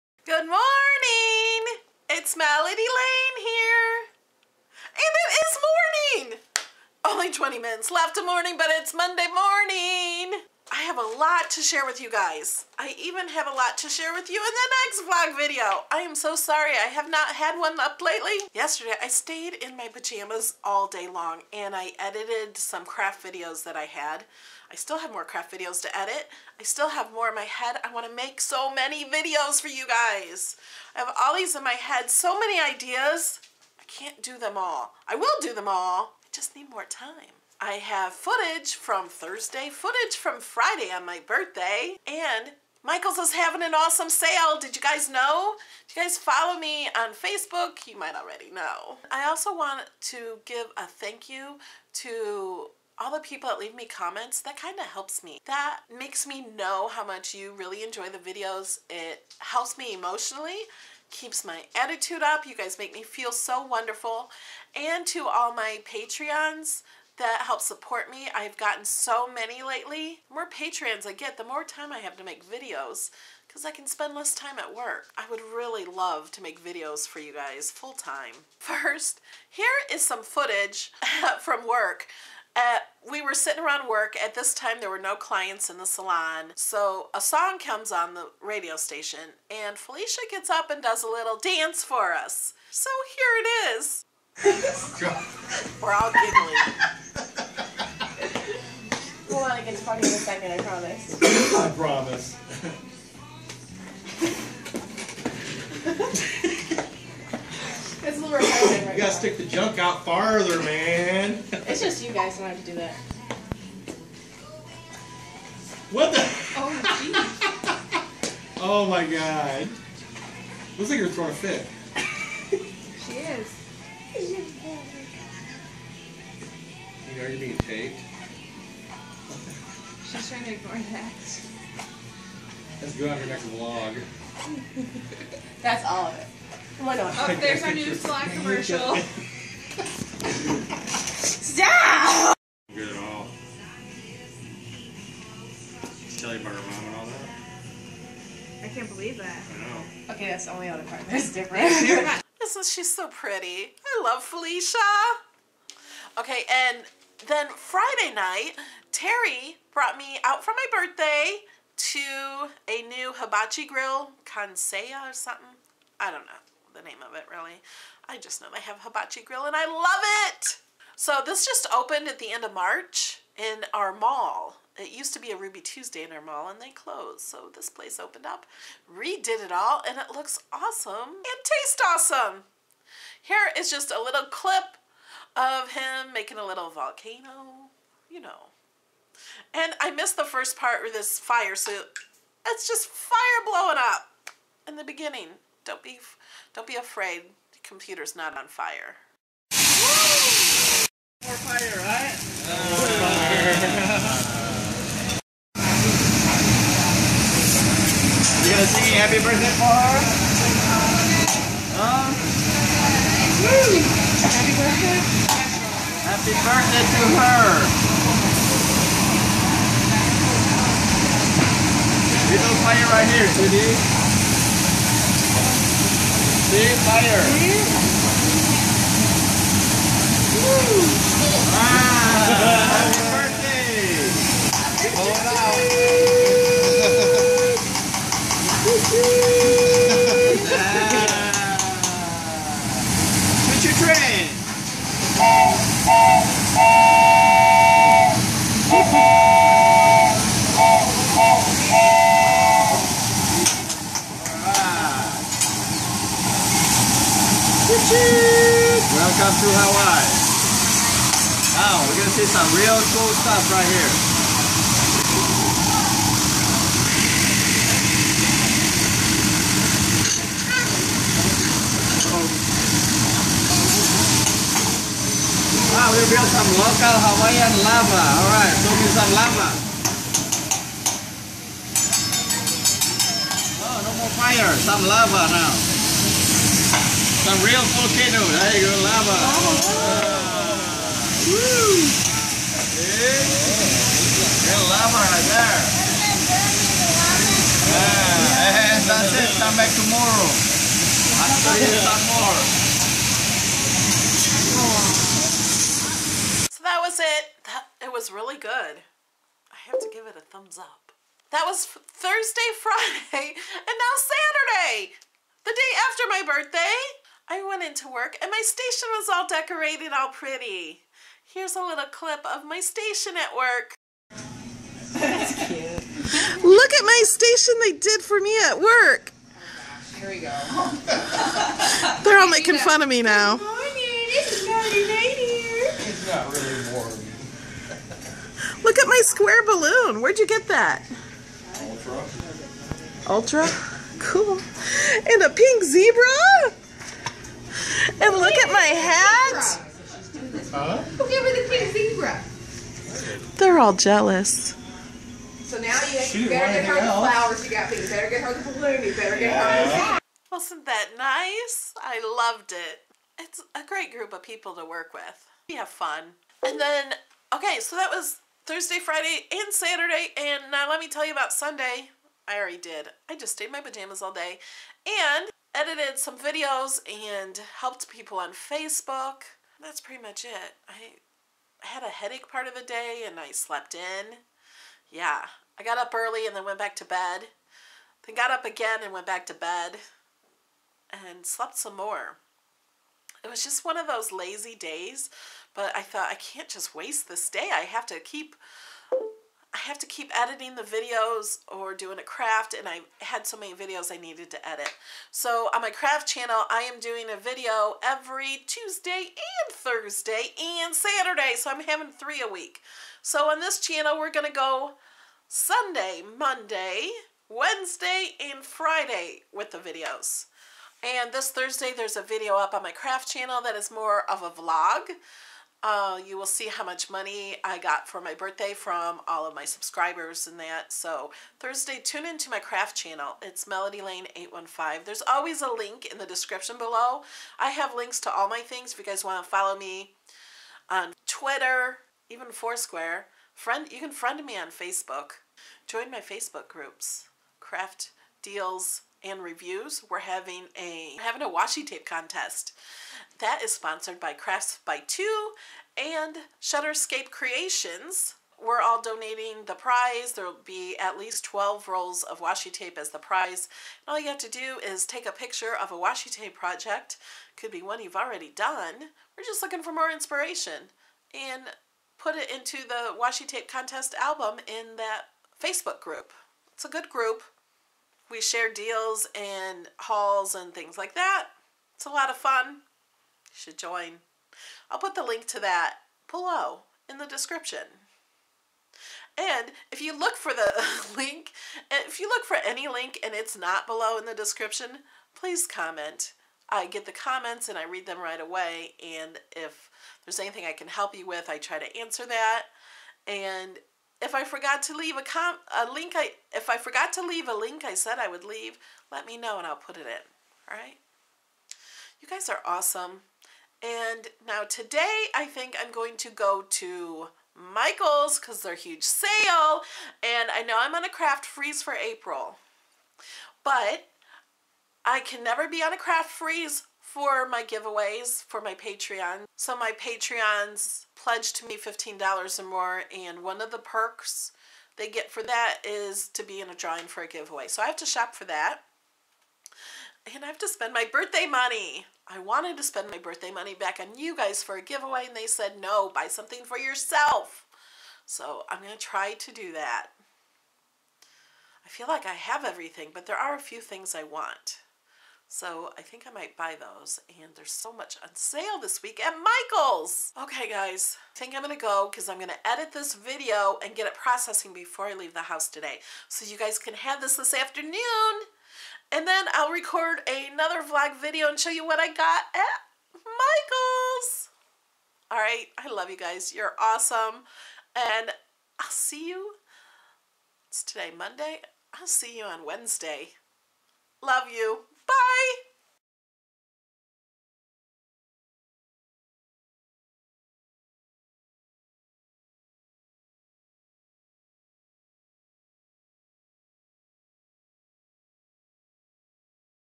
Birthday Dinner & More Vlog #188
Phylicia does a little dance in salon. Had my birthday dinner at Kansai Hibachi grill in Bourbonnais.

Melody Lane
668 N. Convent